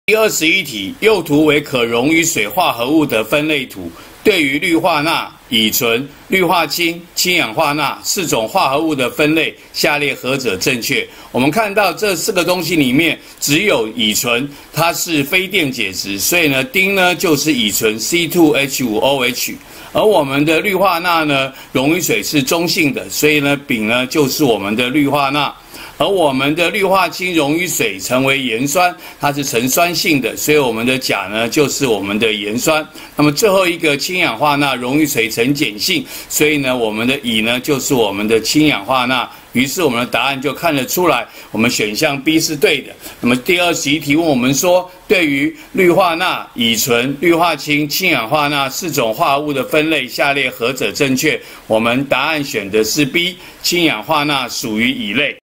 108學年下8年級理化二段第21題
給NaCl、C2H5OH、HCl、NaOH分類圖，問歸類何者正確。